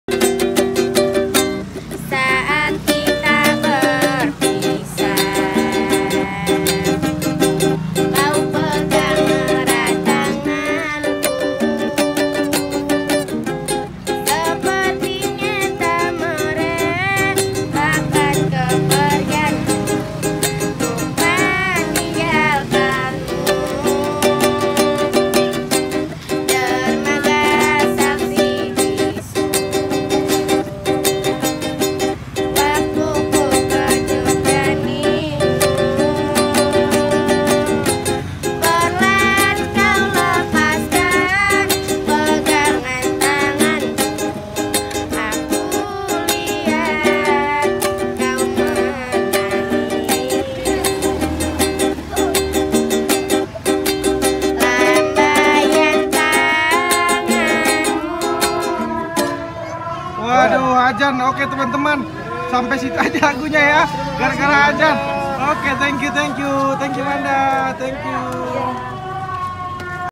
Virall !!! Ketemu Pengamen Keliling Adek Dan Kakak Cantiknya Mirirp Artis Citacita Dan Zaskia Gotik
Pengamen jalanan cantik dan kreatif

Support me :)